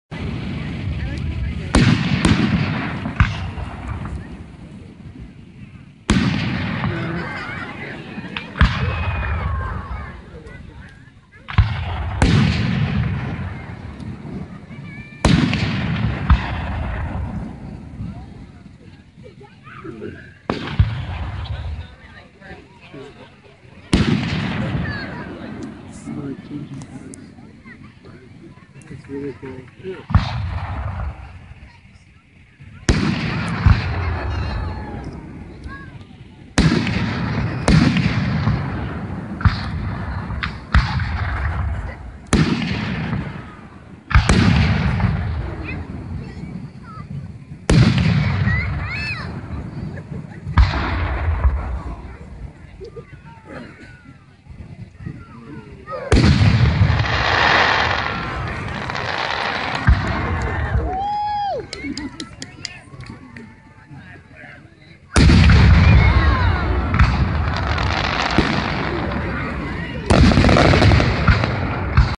Washington CT Fireworks 2014 Part 2
Washington CT Fireworks at Shepaug Valley High School July 5th 2014 Part 2